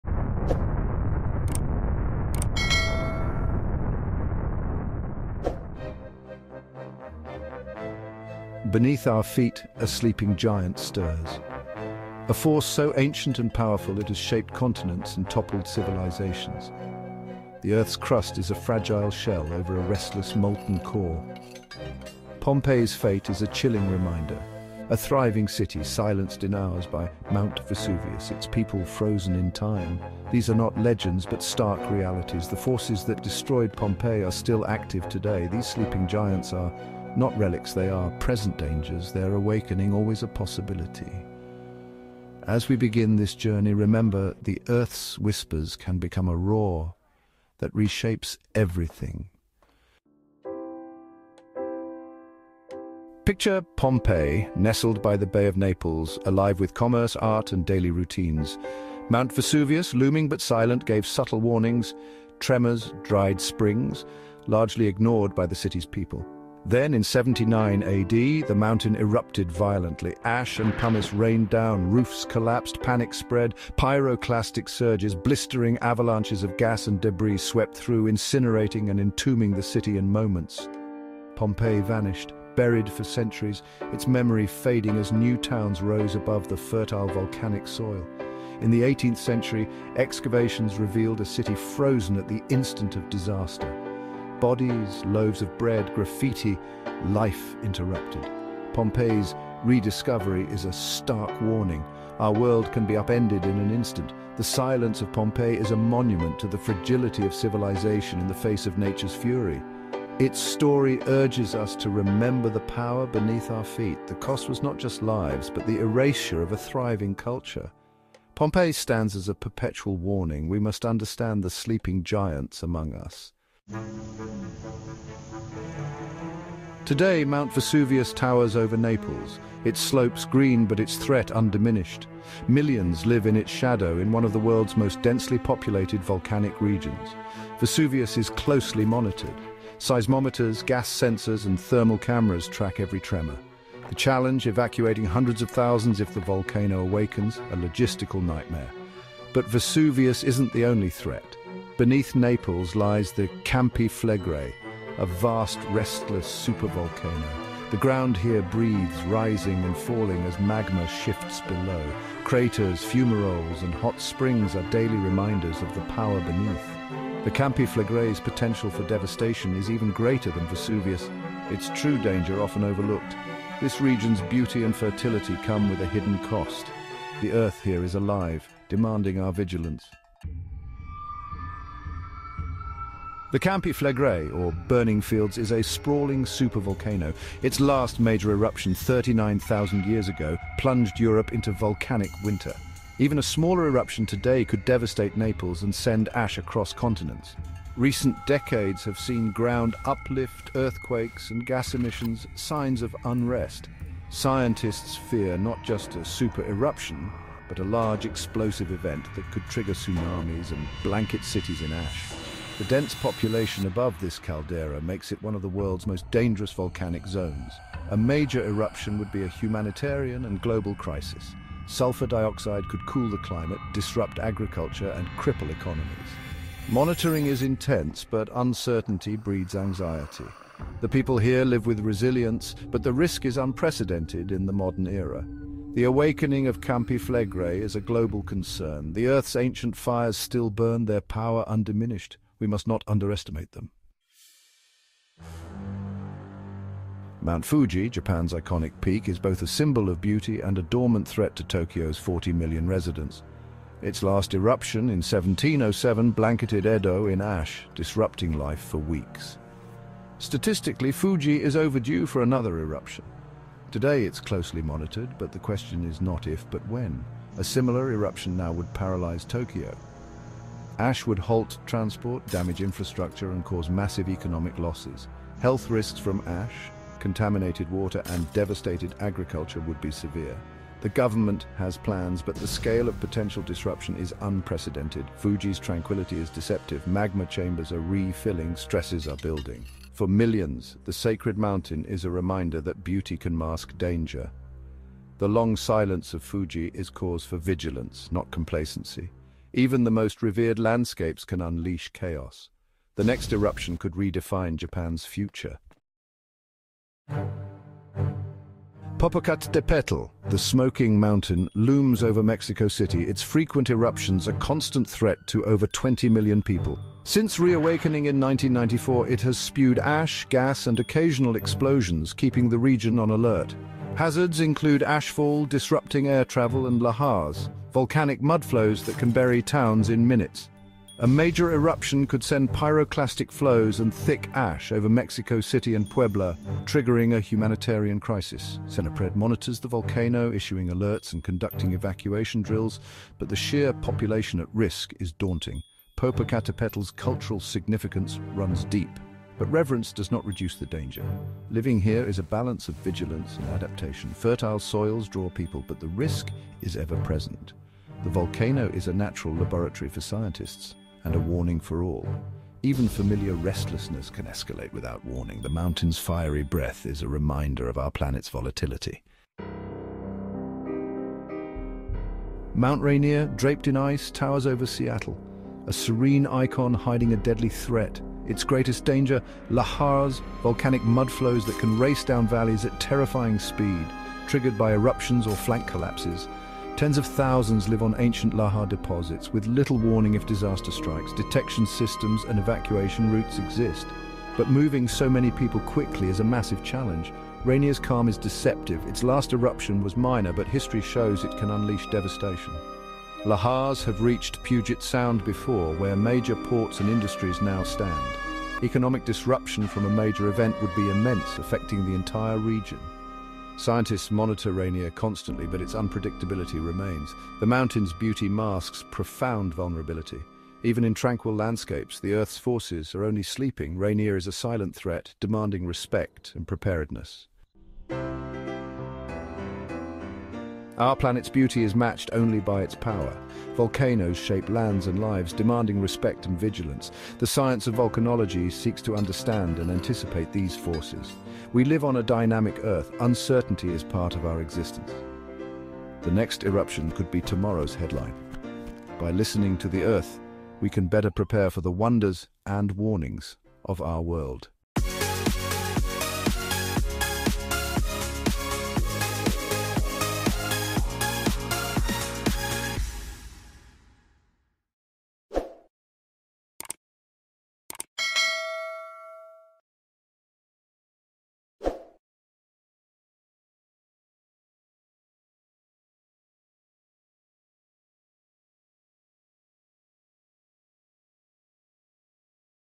Could a Volcano Destroy a City Today? | The Truth About Pompeii, Naples and Supervolcanoes
A volcano destroyed Pompeii in just one day.
Could it happen again… in our time?

This documentary-style video explores the terrifying truth about volcanoes that threaten cities around the world. From the ruins of ancient Pompeii to the modern streets of Naples, we dive deep into the risks posed by Mount Vesuvius, the hidden supervolcano beneath Campi Flegrei, and other sleeping giants like Mount Fuji, Popocatépetl, and Mount Rainier.

🌋 What would happen if Campi Flegrei erupted today?
🌍 Which cities are built next to dangerous volcanoes?
🔬 Can science save us before the next eruption?

👉 If you’re fascinated by natural disasters, ancient history, or the science behind volcanoes, this video is for you.